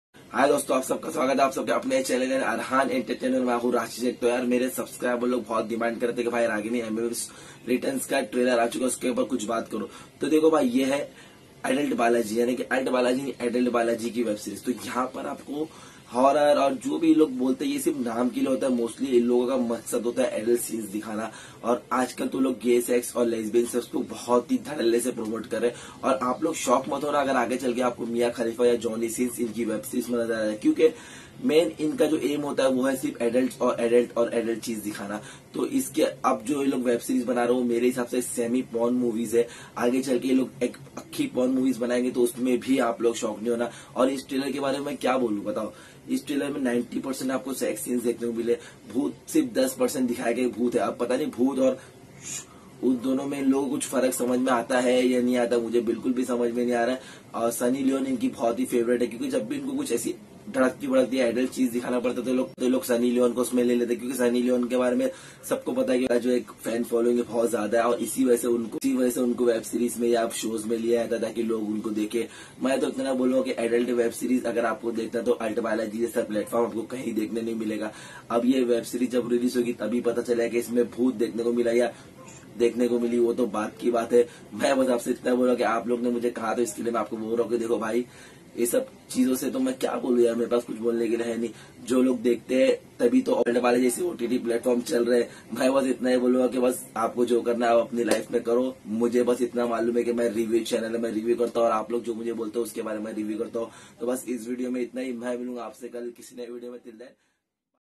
Ragini mms Returns Season 2 Review|Traiiler Review
Hi dosto aaj hum Review karegey Ragini mms Returns Season 2 Review|ALT balaji ki ek Aur chutiya paa wali web Series.
Ragini mms Returns
Ragini mms Returns Season 2
Trailer Review of Ragini mms Returns